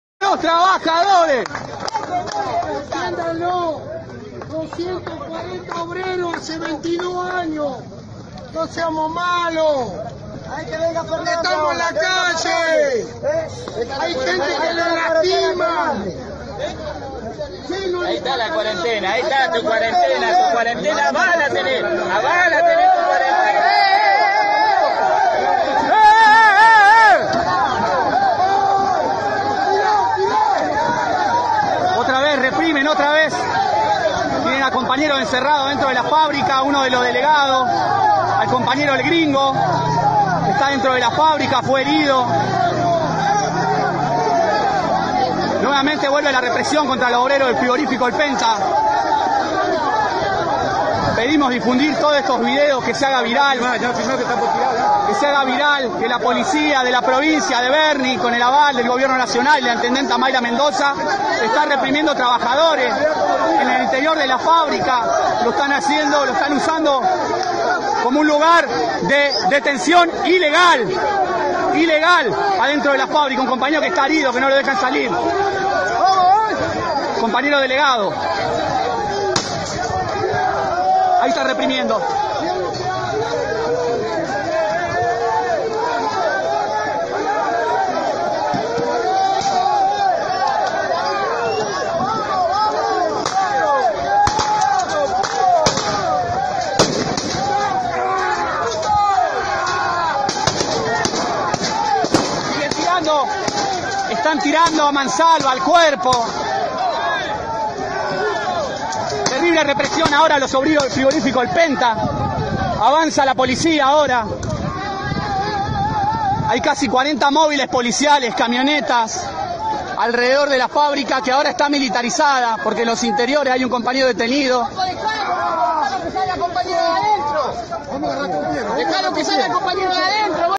Reprimen a trabajadores de un frigorífico de Quilmes e intendenta K cruzó a la Bonaerense
Efectivos de la Policía bonaerense dispersaron con balas de goma a trabajadores que intentaban ingresar a un frigorífico de Quilmes en protesta por 250 despidos y el pago de salarios adeudados. El hecho fue repudiado por el municipio local, que conduce la kirchnerista Mayra Mendoza.